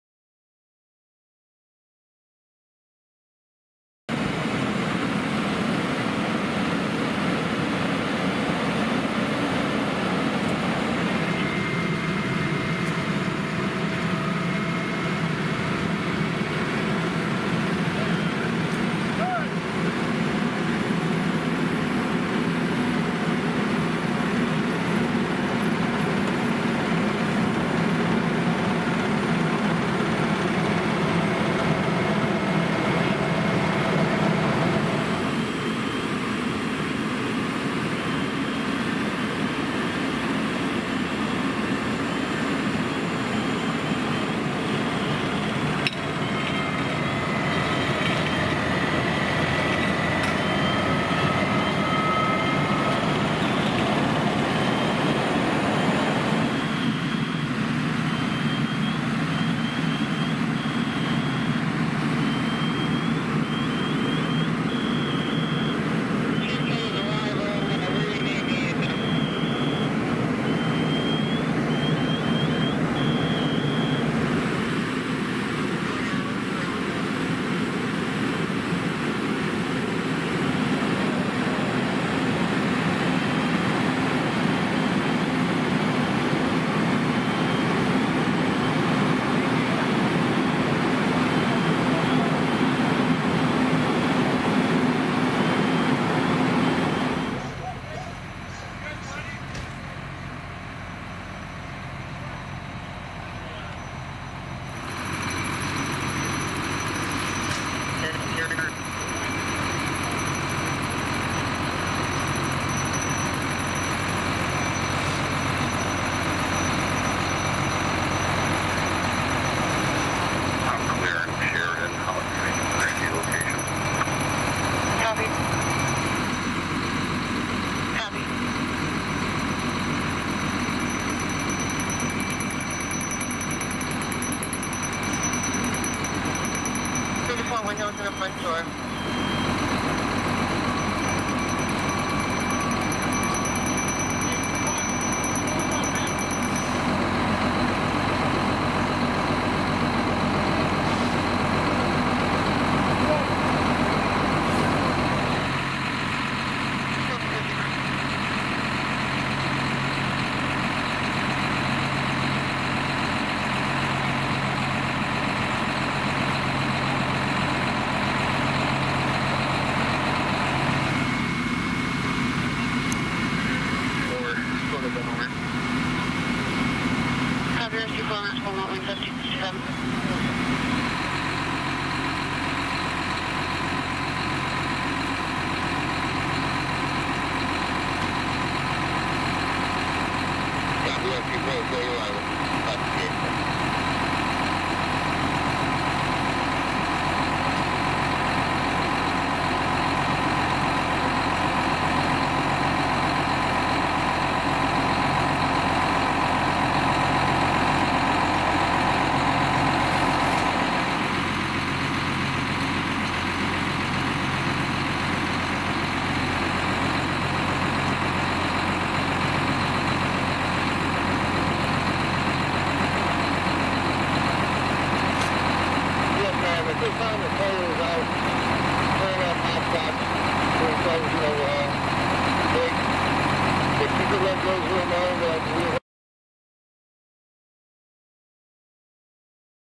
2 House Fires At The Same Time : Garden City and Inkster, Michigan
Crews are currently wrapping up two different house fires in the western Wayne County area. The first fire near Cherry Hill and Merriman Road, in Garden City. Crews were able to knock this down with assistance from Westland and Wayne. Less than 2 miles away, a second fire near Cherry Hill and Middlebelt was handled by Inkster, Westland, and Dearborn Heights Fire Departments.